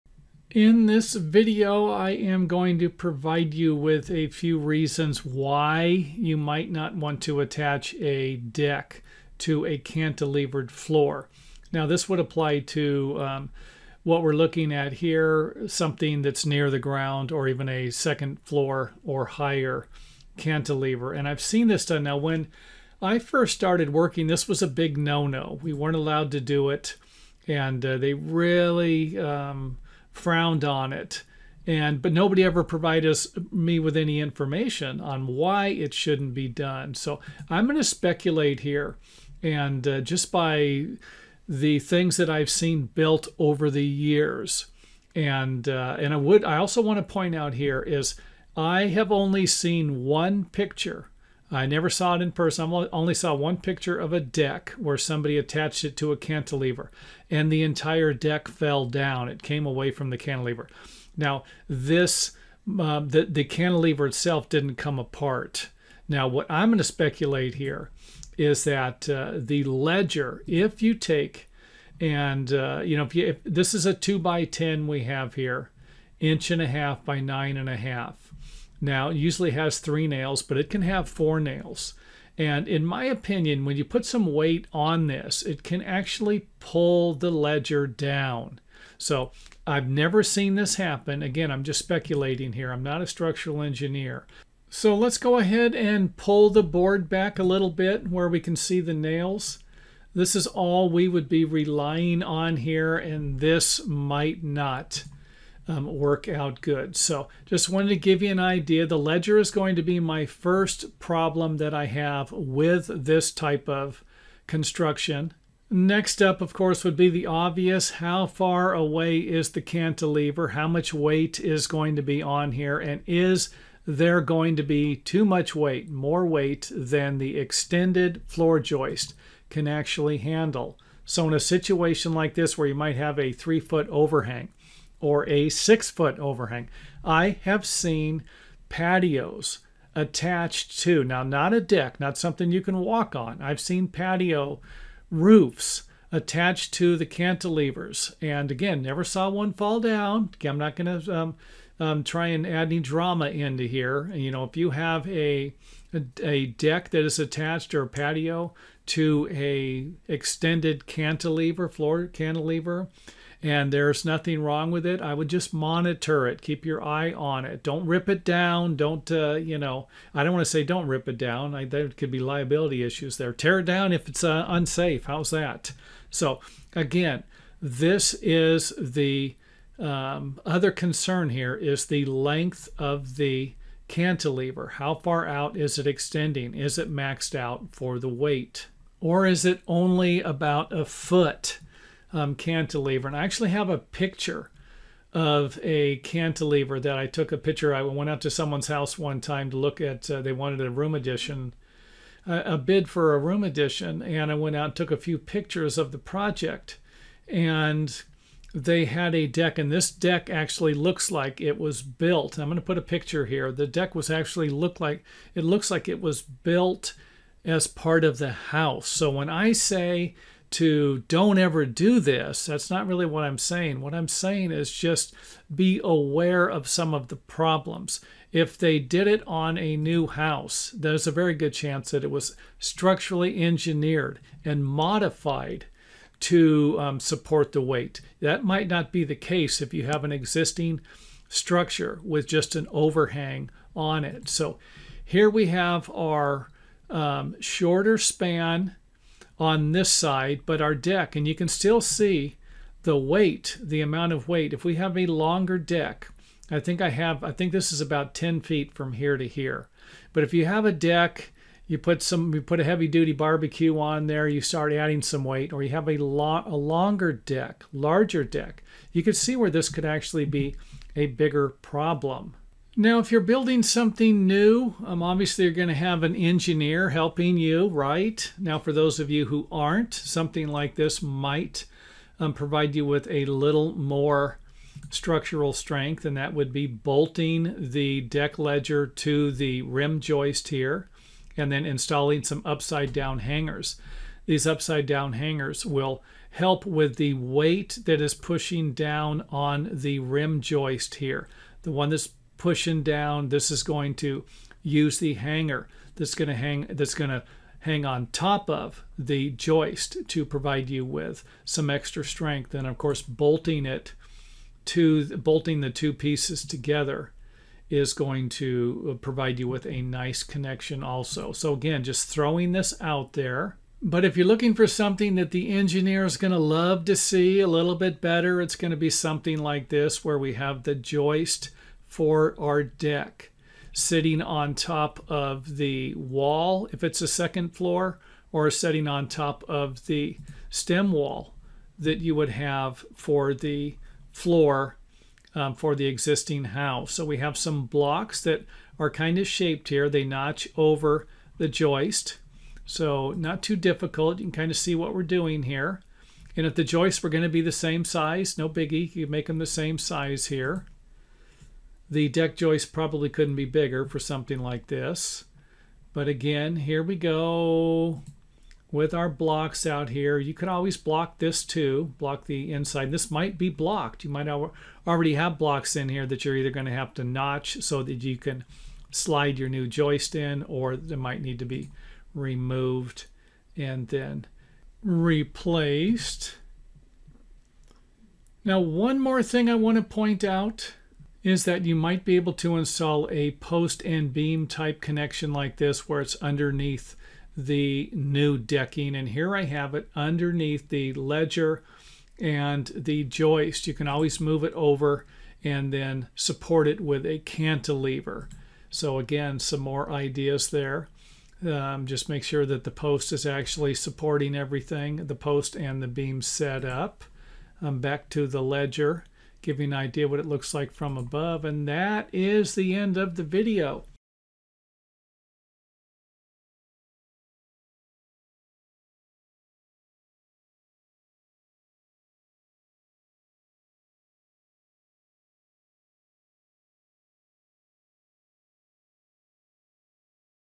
Watch This Video Before You Attach A Deck or Balcony To Floor Cantilever - Structural Framing Tips
Click on this link for more information about new home construction, old house building and floor joists cantilevered framing systems. In this video I will provide you with two problems that could prevent you from attaching a deck, balcony or patio cover to an extended floor joist framed cantilever on an existing house. Structural engineering might also be required for remodeling, renovation and even new house construction projects, along with consulting local building authorities and building departments.